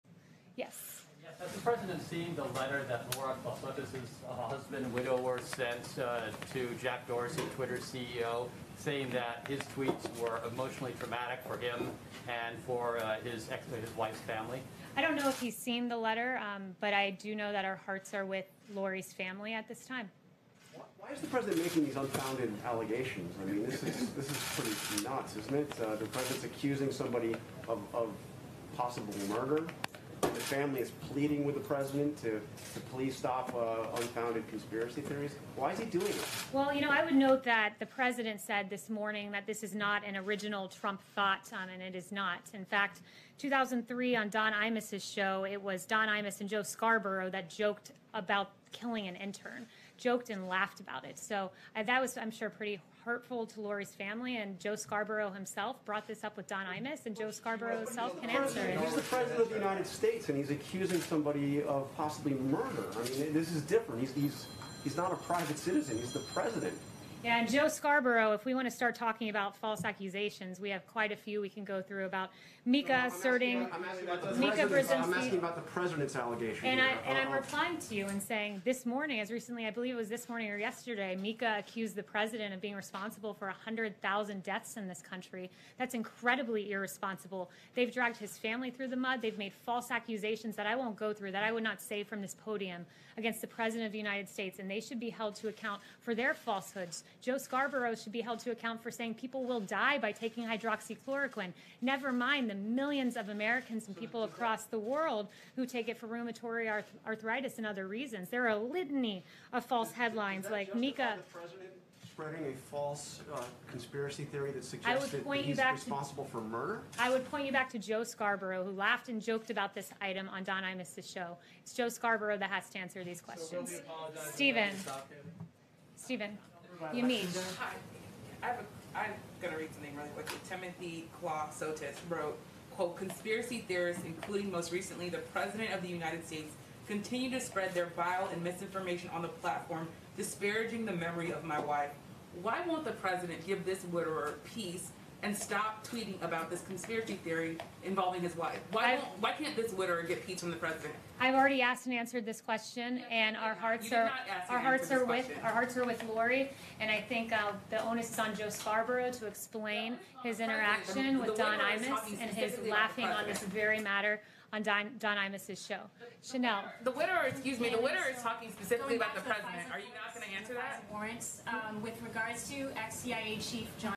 MOMENTS AGO: Kayleigh McEnany spars with reporters over President Trump's attacks on Joe Scarborough
Moments ago, Press Secretary Kayleigh McEnany sparred with reporters over the issue of President Trump's continued attacks on "Morning Joe" host Joe Scarborough.